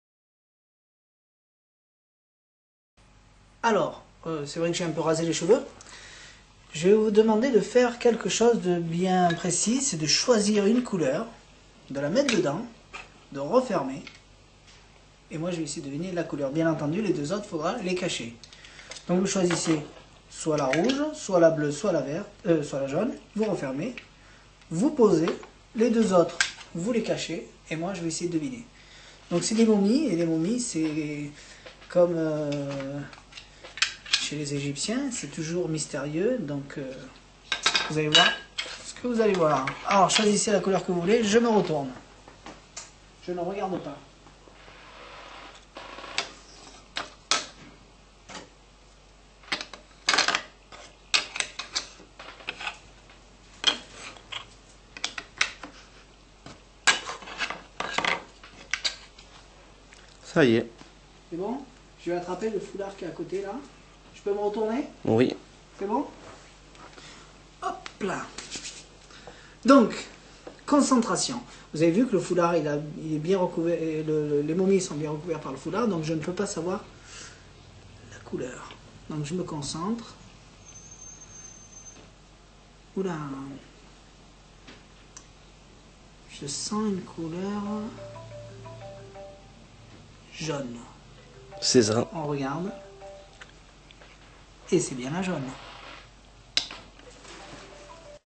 les momies - le petit magicien
le spectateur cache une momie dans son sarcophage. Et aprés une grosse concentration, le magicien devine la couleur de la momie